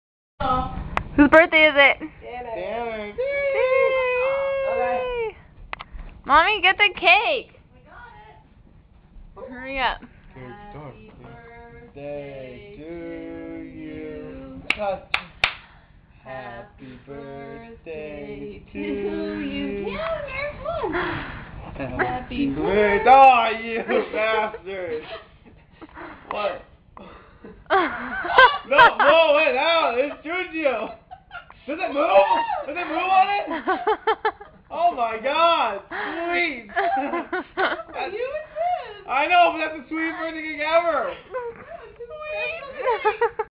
Tanner's Bday surprise
Tim is sadly surprised tanner recieves a thomas the tank engine birthday cake on HIS birthday.